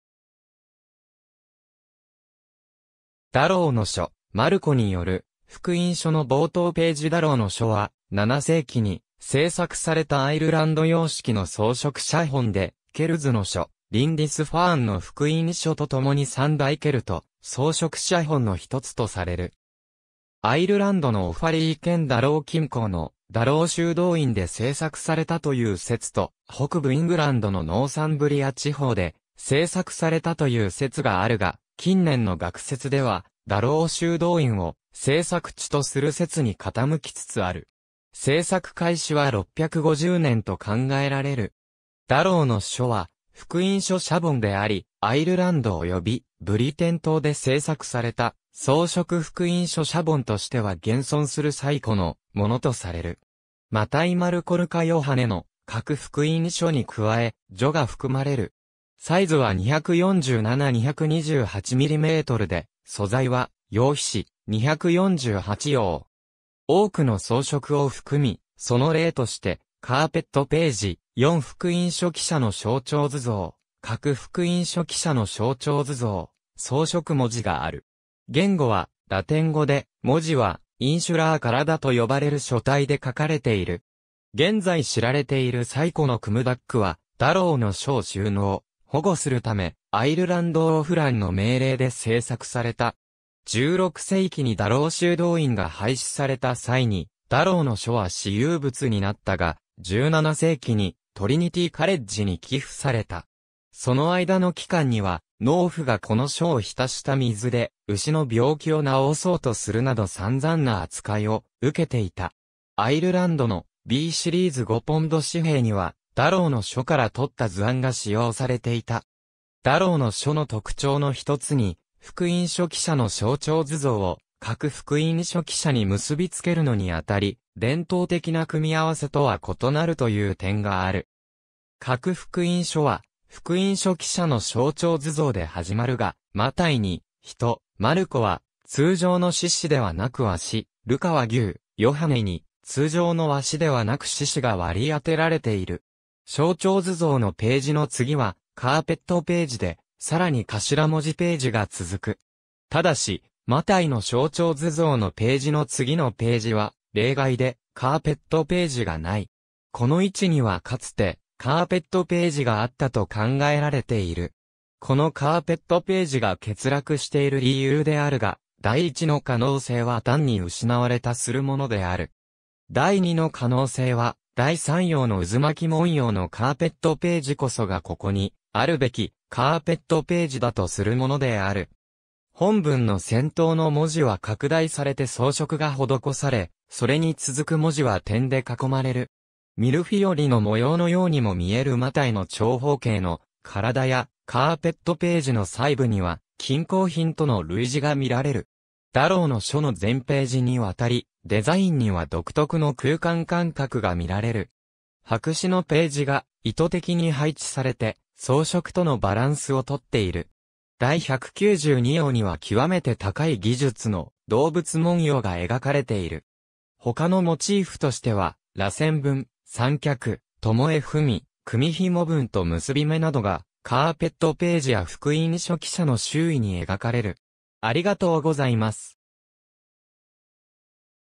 ダロウの書
ダロウの書、マルコによる福音書の冒頭頁 ダロウの書（ダロウのしょ、英語: Book of Durrow、ダブリン・トリニティカレッジ図書館蔵、MS A. 4. 5. （57））は、7世紀に制作されたアイルランド様式の装飾写本で「ケルズの書」、「リンディスファーンの福音書」とともに三大ケルト装飾写本のひとつとされる。
アイルランドのオファリー県ダロウ近郊のダロウ修道院で制作されたという説と、北部イングランドのノーサンブリア地方で制作されたという説があるが、近年の学説ではダロウ修道院を制作地とする説に傾きつつある。
制作開始は650年と考えられる。
ダロウの書は福音書写本であり、アイルランドおよびブリテン島で制作された装飾福音書写本としては現存する最古のものとされる。
マタイ・マルコ・ルカ・ヨハネの各福音書に加え、序が含まれる。
サイズは247×228mmで、素材は羊皮紙、248葉。
多くの装飾を含み、その例としてカーペット頁（現存するのは6頁）、四福音書記者の象徴図像（1頁）、各福音書記者の象徴図像（福音書記者ごとに1頁で計4頁）、装飾文字（6頁）がある。
言語はラテン語で、文字はインシュラー体（島の文字、Insular Script）と呼ばれる書体で書かれている。
現在知られている最古のクムダック（聖書を収める箱）は、ダロウの書を収納・保護するため、アイルランド王フラン（Flann Sinna、879-916）の命令で製作された。
16世紀にダロウ修道院が廃止された際にダロウの書は私有物になったが、17世紀にトリニティカレッジに寄付された。
その間の期間には、農夫がこの書を浸した水で牛の病気を治そうとするなど散々な扱いを受けていた。
アイルランドの「Bシリーズ」5ポンド紙幣にはダロウの書から採った図案が使用されていた。
ダロウの書の特徴のひとつに、福音書記者の象徴図像を各福音書記者に結びつけるのにあたり、伝統的な組み合わせとは異なるという点がある。
各福音書は福音書記者の象徴図像ではじまるが、マタイに人、マルコは通常の獅子ではなく鷲、ルカは牛、ヨハネに通常の鷲ではなく獅子が割り当てられている。
象徴図像の頁の次はカーペット頁で、さらに頭文字頁が続く。
ただしマタイの象徴図像（人）の頁の次の頁は例外で、カーペット頁がない。
この位置にはかつてカーペット頁があったと考えられている。
このカーペット頁が欠落している理由であるが、第一の可能性はたんに失われたするものである。
第二の可能性は、第3葉の渦巻文様のカーペット頁こそがここにあるべきカーペット頁だとするものである。
本文の先頭の文字は拡大されて装飾が施され、それに続く文字は点で囲まれる。
ミルフィオリの模様のようにも見えるマタイの長方形の体やカーペット頁の細部には、金工品との類似が見られる。
ダロウの書の全頁にわたり、デザインには独特の空間感覚が見られる。
白紙のページが意図的に配置されて装飾とのバランスをとっている。
第192葉には極めて高い技術の動物文様が描かれている。
他のモチーフとしては、螺旋文、三脚巴文、組紐文と結び目などが、カーペット頁や福音書記者の周囲に描かれる。